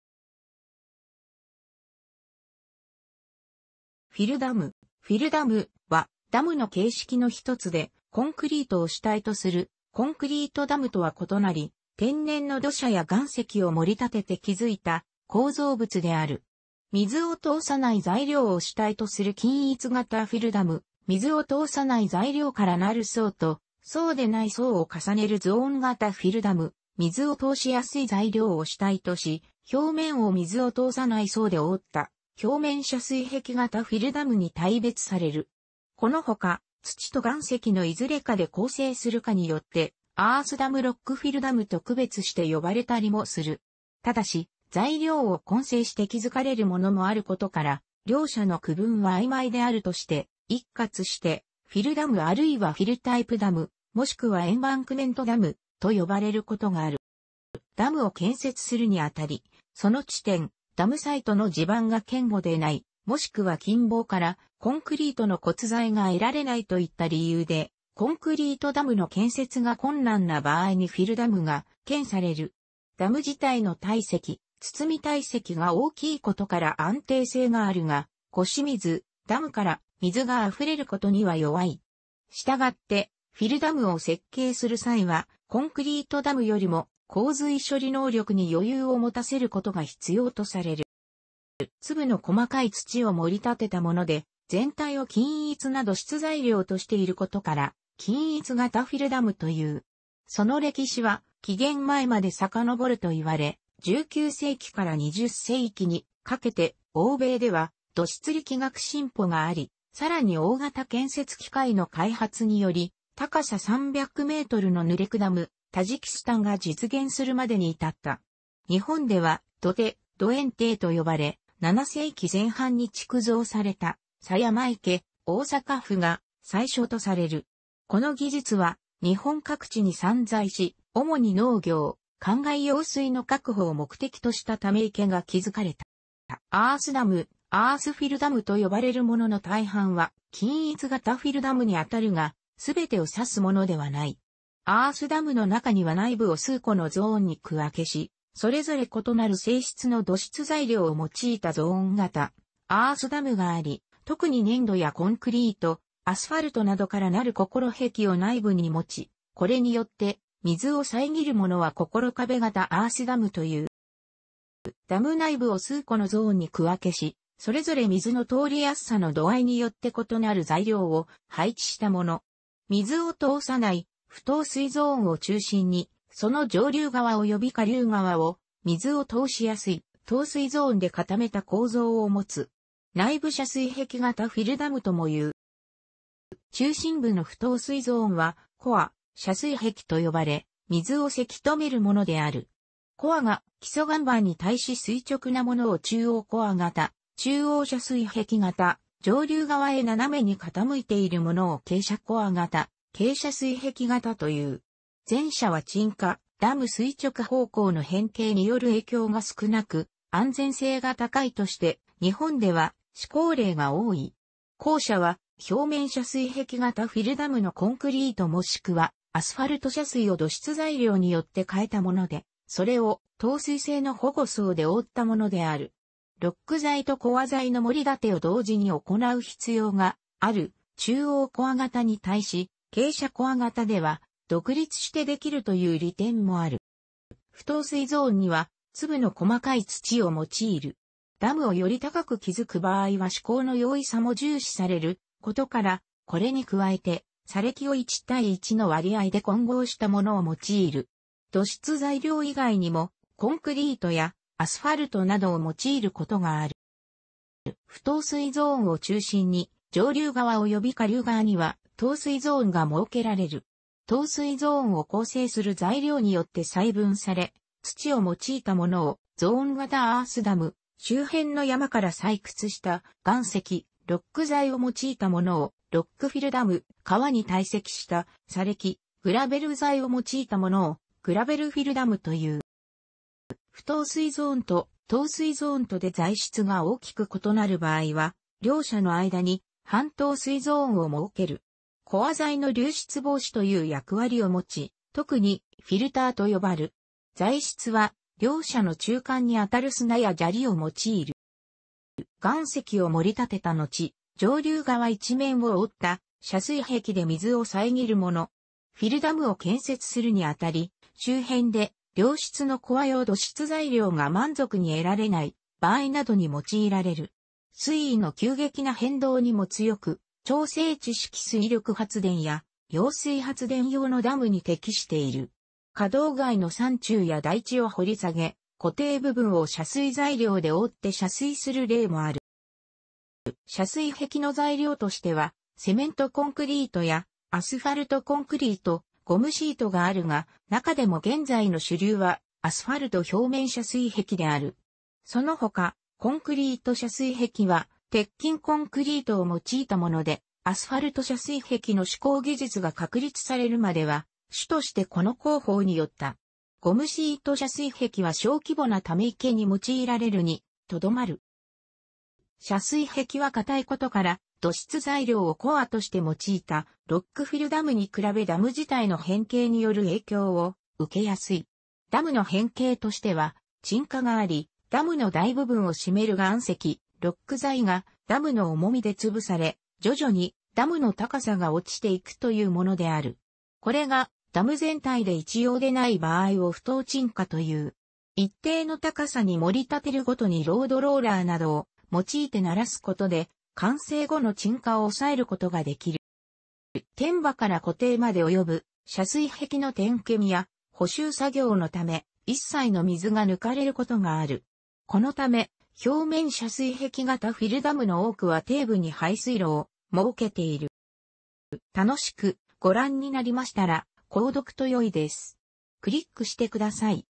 フィルダム
フィルダム（）は、ダムの型式の一つで、コンクリートを主体とするコンクリートダムとは異なり、天然の土砂や岩石を盛り立てて築いた構造物である。水を通さない材料を主体とする均一型フィルダム・水を通さない材料からなる層と、そうでない層を重ねるゾーン型フィルダム・水を通しやすい材料を主体とし、表面を水を通さない層で覆った表面遮水壁型フィルダムに大別される。このほか、土と岩石のいずれかで構成するかによってアースダム・ロックフィルダムと区別して呼ばれたりもする。ただし、材料を混成して築かれるものもあることから、両者の区分はあいまいであるとして、一括してフィルダムあるいはフィルタイプダム、もしくはエンバンクメントダム（）と呼ばれることがある。

ダムを建設するにあたり、その地点（ダムサイト）の地盤が堅固でない、もしくは近傍からコンクリートの骨材が得られないといった理由でコンクリートダムの建設が困難な場合にフィルダムが検討される。ダム自体の体積（堤堆積）が大きいことから安定性があるが、越水（ダムから水があふれること）には弱い。したがってフィルダムを設計する際は、コンクリートダムよりも洪水処理能力に余裕を持たせることが必要とされる。

粒の細かい土を盛り立てたもので、全体を均一な土質材料としていることから均一型フィルダムという。その歴史は紀元前までさかのぼるといわれ、19世紀から20世紀にかけて欧米では土質力学の進歩があり、さらに大型建設機械の開発により、高さ300メートルのヌレクダム（タジキスタン）が実現するまでに至った。日本では土手（どて）・土堰堤（どえんてい）と呼ばれ、7世紀前半に築造された狭山池（大阪府）が最初とされる。この技術は日本各地に散在し、主に農業（かんがい）用水の確保を目的としたため池が築かれた。

アースダム（アースフィルダム）と呼ばれるものの大半は均一型フィルダムにあたるが、すべてを指すものではない。アースダムの中には内部を数個のゾーンに区分けし、それぞれ異なる性質の土質材料を用いたゾーン型アースダムがあり、特に粘土やコンクリート、アスファルトなどからなる心壁を内部に持ち、これによって水を遮るものは心壁型アースダムという。

ダム内部を数個のゾーンに区分けし、それぞれ水の通りやすさの度合いによって異なる材料を配置したもの。「水を通さない」不透水ゾーンを中心に、その上流側および下流側を「水を通しやすい」透水ゾーンで固めた構造をもつ。内部遮水壁型フィルダムともいう。

中心部の不透水ゾーンはコア（遮水壁）と呼ばれ、水をせき止めるものである。コアが基礎岩盤に対し垂直なものを中央コア型（中央遮水壁型）、上流側へ斜めに傾いているものを傾斜コア型（傾斜遮水壁型）という。前者は沈下（ダム垂直方向の変形）による影響が少なく、安全性が高いとして日本では施工例が多い。後者は表面遮水壁型フィルダムのコンクリートもしくはアスファルト遮水壁を土質材料によって代えたもので、それを透水性の保護層で覆ったものである。ロック材とコア材の盛り立てを同時に行う必要がある中央コア型に対し、傾斜コア型では独立してできるという利点もある。

不透水ゾーンには粒の細かい土を用いる。ダムをより高く築く場合は施工の容易さも重視されることから、これに加えて砂れきを1対1の割合で混合したものを用いる。土質材料以外にもコンクリートやアスファルトなどを用いることがある。

不透水ゾーンを中心に上流側および下流側には透水ゾーンが設けられる。透水ゾーンを構成する材料によって細分され、土を用いたものをゾーン型アースダム、周辺の山から採掘した岩石（ロック材）を用いたものをロックフィルダム、川に堆積した砂れき（グラベル材）を用いたものをグラベルフィルダムという。

不透水ゾーンと透水ゾーンとで材質が大きく異なる場合は、両者の間に半透水ゾーンを設ける。コア材の流出防止という役割を持ち、特にフィルターと呼ばる。材質は両者の中間にあたる砂や砂利を用いる。

岩石を盛り立てたのち、上流側一面を覆った遮水壁で水を遮るもの。フィルダムを建設するにあたり、周辺で良質のコア用土質材料が満足に得られない場合などに用いられる。水位の急激な変動にも強く、調整池式水力発電や揚水発電用のダムに適している。河道外の山中や台地を掘り下げ、湖底部分を遮水材料で覆って遮水する例もある。

遮水壁の材料としてはセメントコンクリートやアスファルトコンクリート、ゴムシートがあるが、中でも現在の主流はアスファルト表面遮水壁である。そのほか、コンクリー...